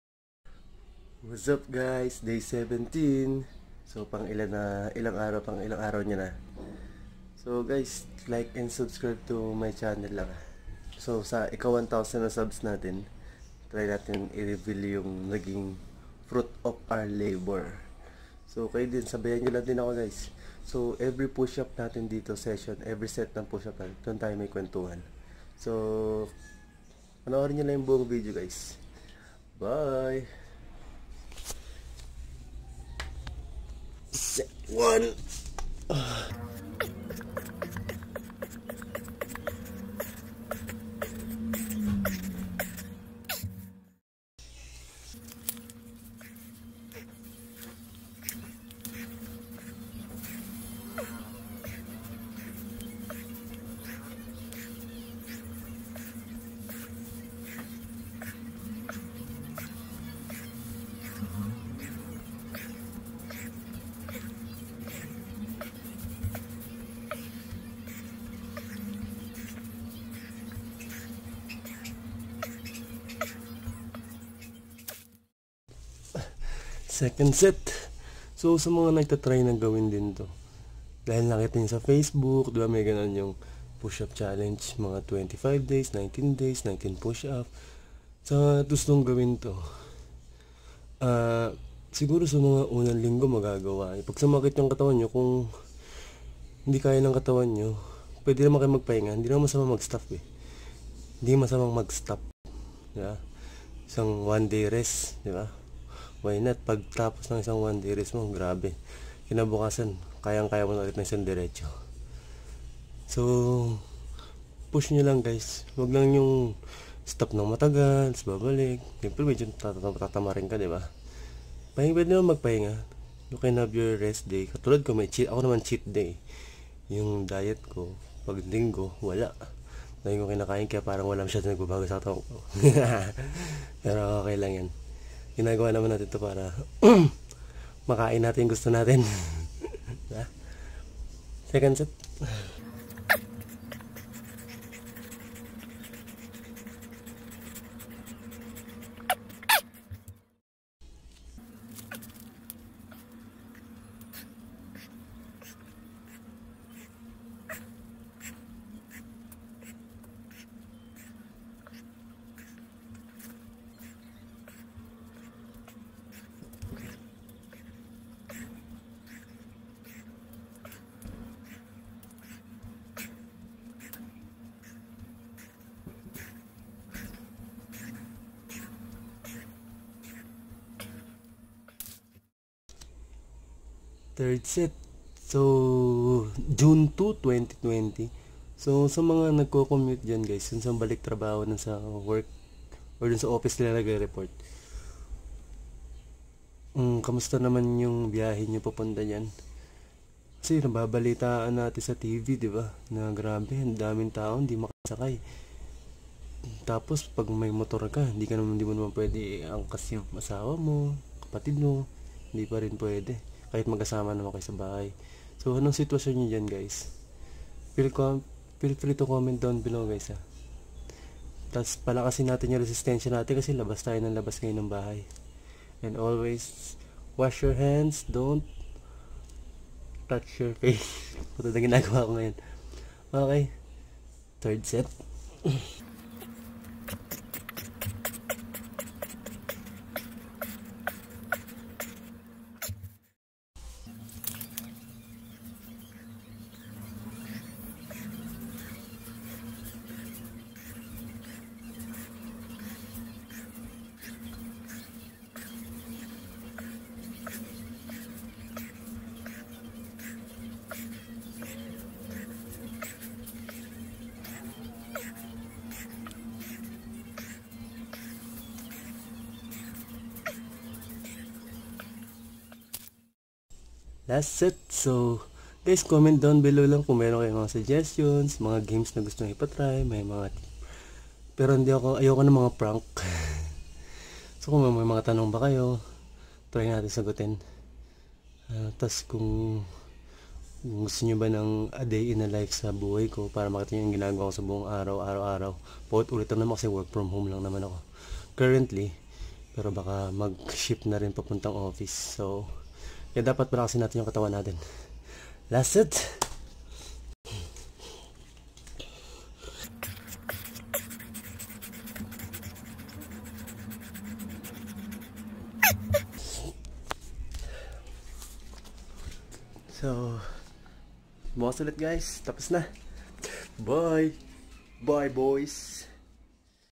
Day 17 | 100 push ups and sit ups (Pinoy challenge)
Day 17
25reps 4 sets - push ups
40reps 3 sets - sit ups

**still doing my whole body exercise, video on day 2
**Will show the result after 1000 reaching subs - currently doing intermittent fasting(I just eat what i want in certain span of time)